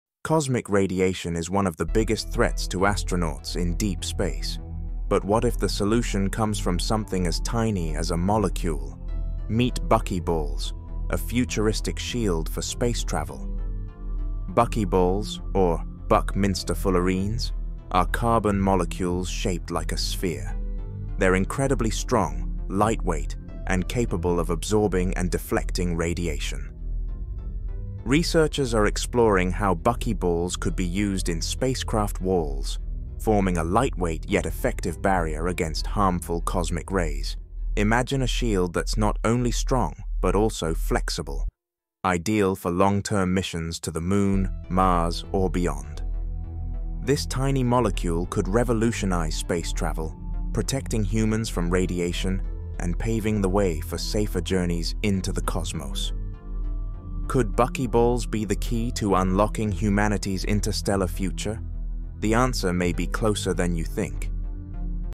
Buckyballs
🌌 Buckyballs: The Future of Space Travel Protection? 🛡️
Cosmic radiation is one of the greatest challenges for space travel, but a tiny molecule might hold the key to solving it! Meet buckyballs—incredibly strong, lightweight carbon molecules that could revolutionize shielding in spacecraft.

In this short, discover how these spherical molecules can:

✨ Absorb and deflect harmful cosmic rays.
✨ Create lightweight and flexible protective barriers.
✨ Pave the way for safer missions to the Moon, Mars, and beyond!
Are buckyballs the secret to humanity's interstellar future? Watch now to explore the possibilities! 🚀